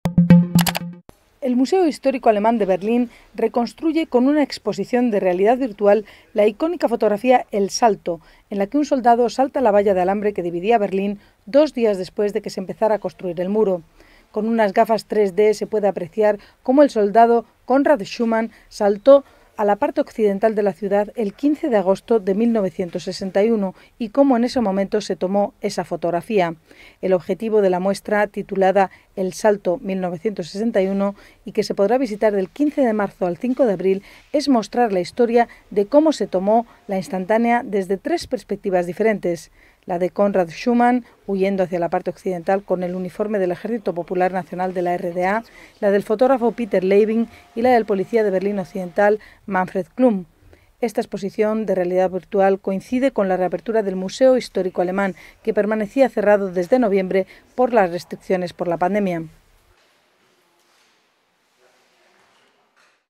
"El Salto", icónica foto del Muro de Berlín, reconstruida en realidad virtual
Berlin (Alemania), 11 mar (EFE/EPA).- (Imagen: Clemens Bilan) La exposición "The Leap - 1961" (El Salto - 1961), que se inaugura en Berlín el próximo 15 de marzo,  repasa con una instalación de realidad virtual el icónico momento en que un policía de Alemania del Este escapa al oeste saltando una valla de alambre de espino.
Elagente Conrad Schumann, de 19 años, huyó así el 15  de agosto de 1961 desde la República Democrática Alemana hacia el sector francés de Berlín Occidental, un momento inmortalizado por el reportero gráfico alemán Peter Leibing.
La instalación se exhibe en el Museo de Historia Alemana en Berlin hasta el 5 de abril.
INCLUYE IMÁGENES DE RECURSO DE LA EXPOSICIÓN "THE LEAP" DURANTE EL PASE DE PRENSA.